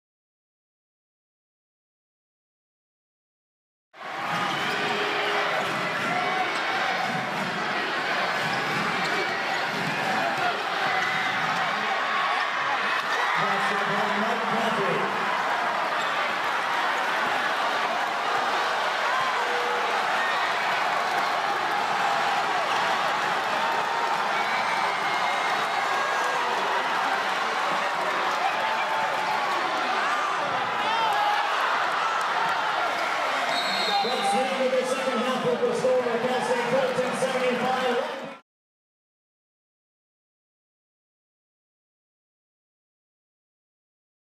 Mike Caffey sends game to OT for Long Beach State
Mike Caffey led a comeback against Cal State Fullerton, scoring a career-high 34 points and hit floater with 17 seconds in regulation to send the game to OT. Long Beach State won 91-85.